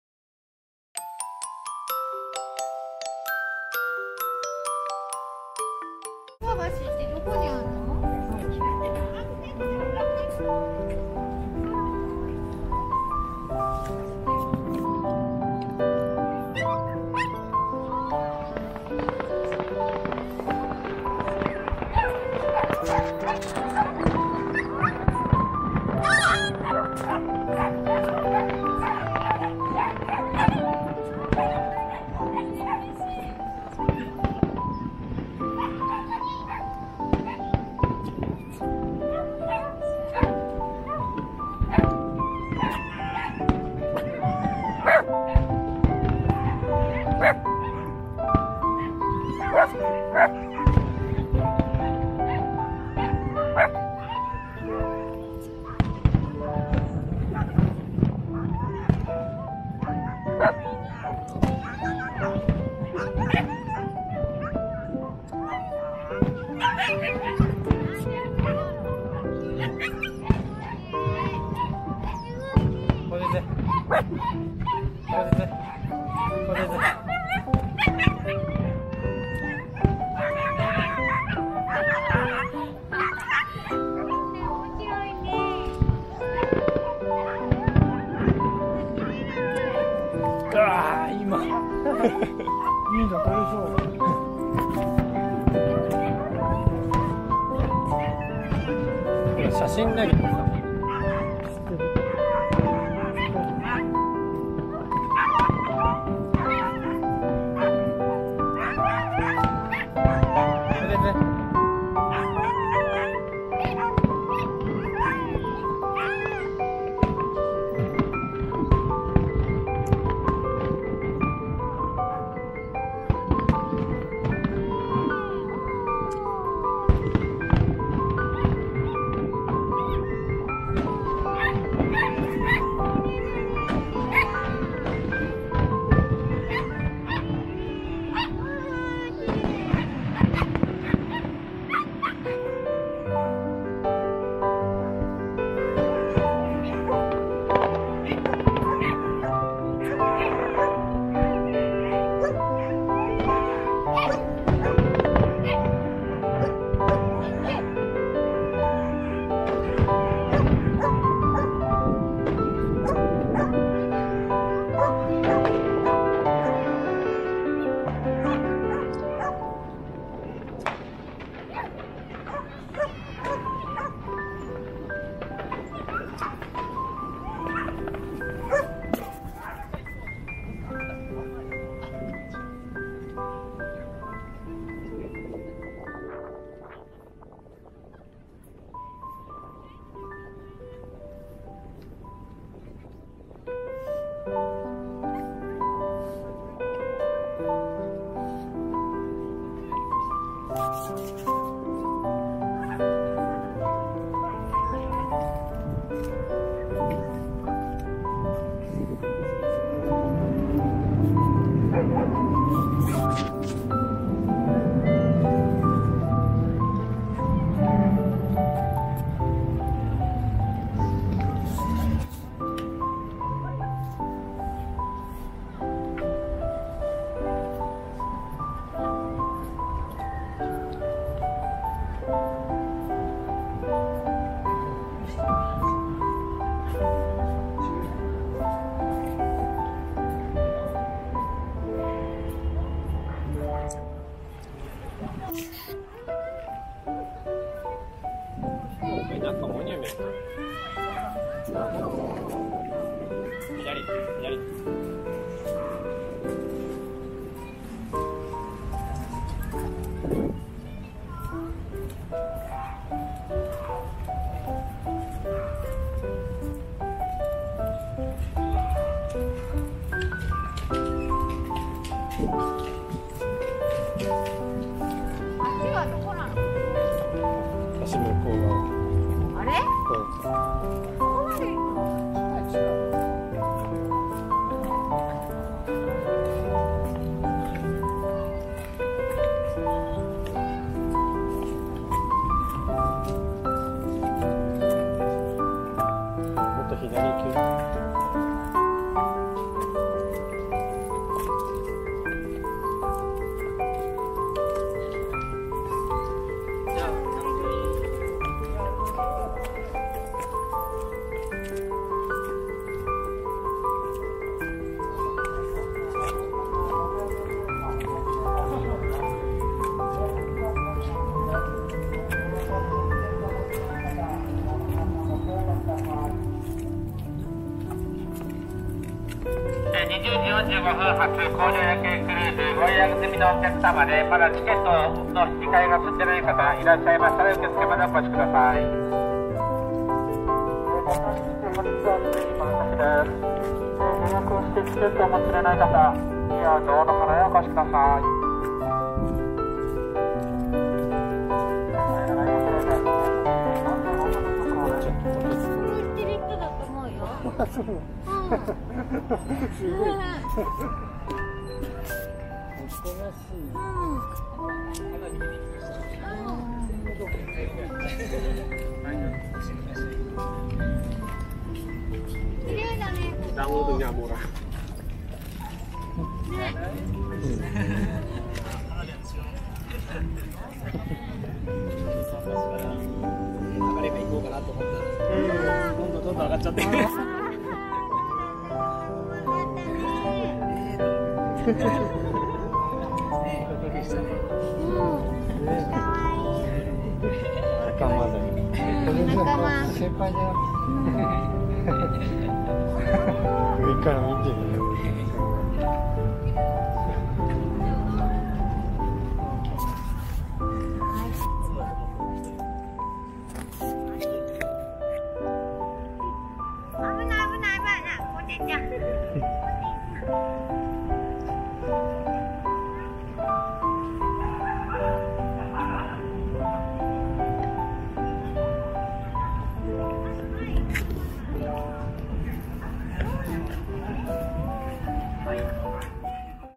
【イヌヨイチ】に行ったあとは花火を見ました（パピヨンとコーギーのミックス犬）After going to [Inuyoichi], I saw fireworks
前回の動画の続きになります。イヌヨイチにいったあとは花火を見ました。
少し暗い動画になりますが、ご勘弁を